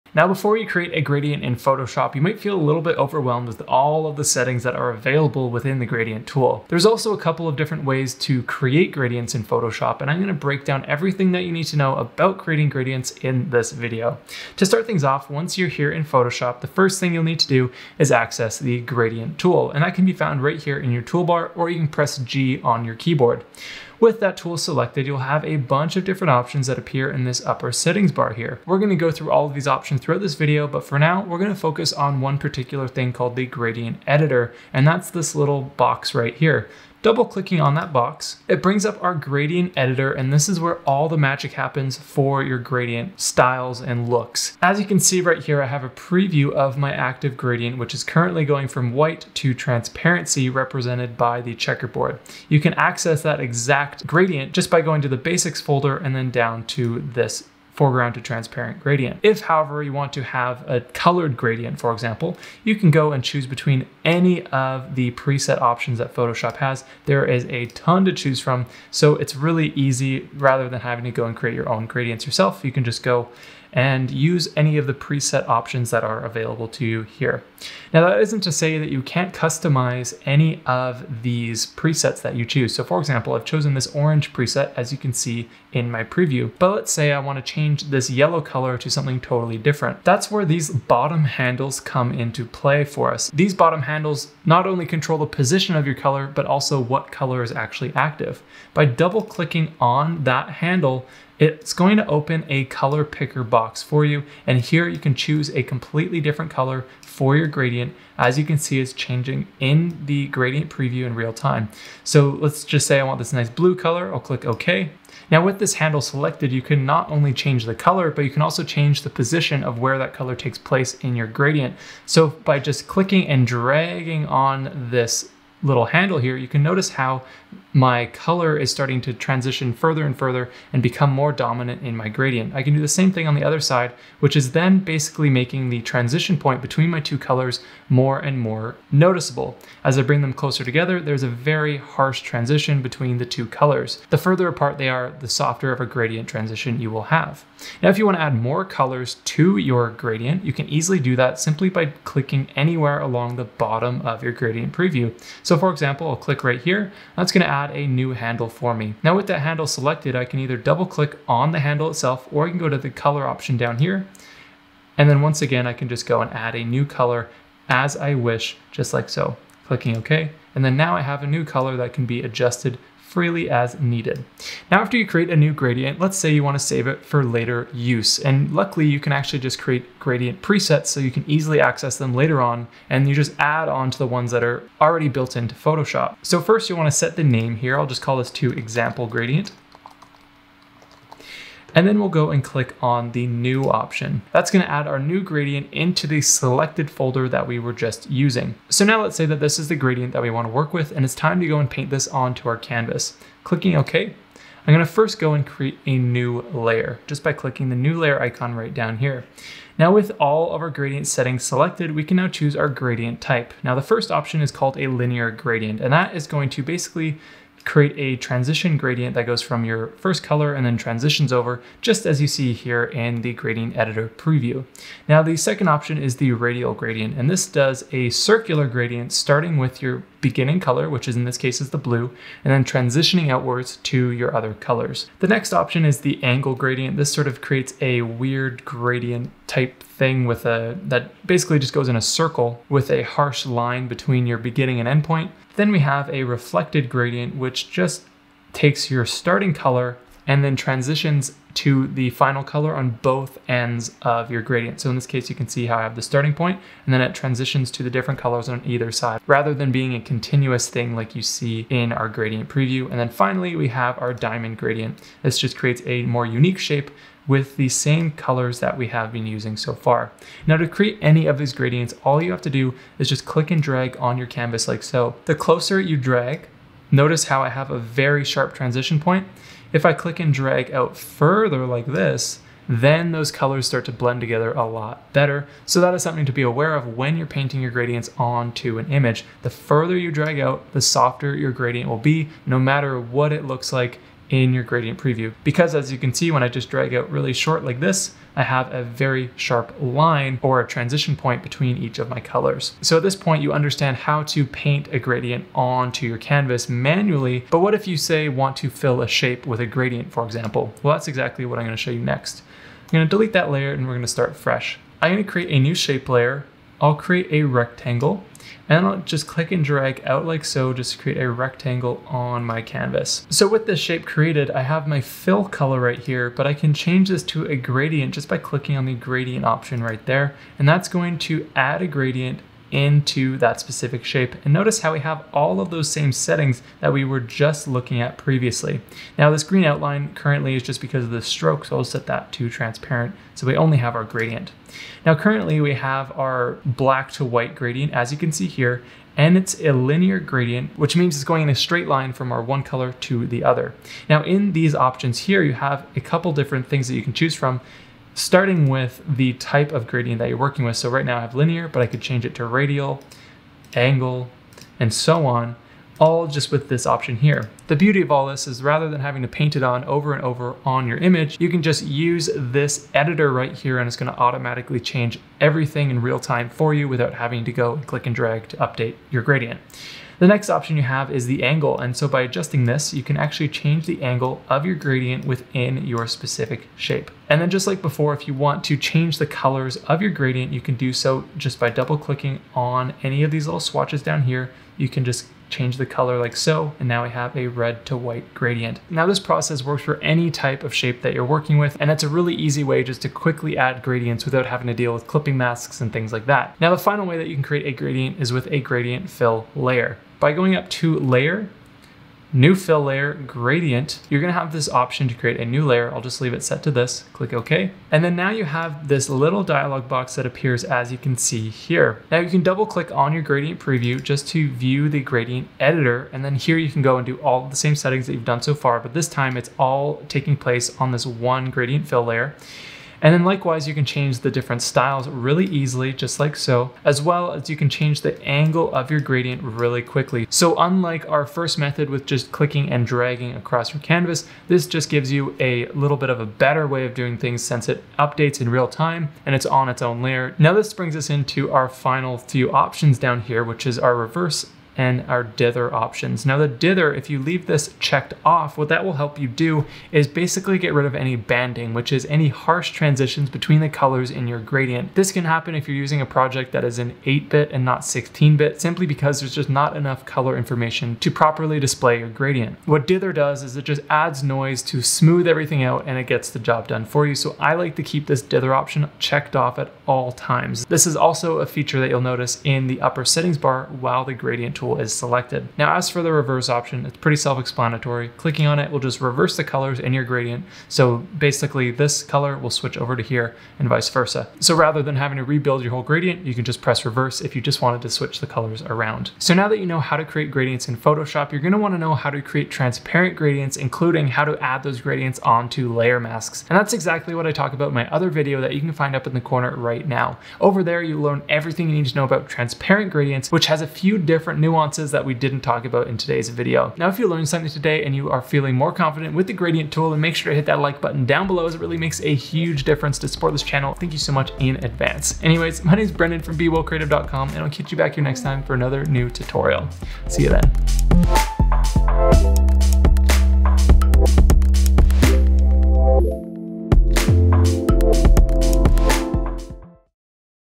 How To Use The Gradient Tool In Photoshop
There are many different ways to use the gradient tool in Photoshop to add colored gradients to your images with ease. Whether you want to paint directly on a layer, fill a shape with a gradient, or use gradient fill layers, there are many options to suite your style. By the end of this photoshop tutorial you'll know how to confidently create and use gradients in each of these 3 methods.

Timestamps
0:00 Creating A Basic Gradient In Photoshop
1:21 Customizing Gradients
2:33 Adding Colors To Gradients
3:00 Creating Gradient Presets
3:31 Adding Gradients To Canvas
5:10 Filling Shapes With Gradients
6:48 Using Gradient Fill Layers
7:40 Additional Gradient Settings
8:37 Wrapping Up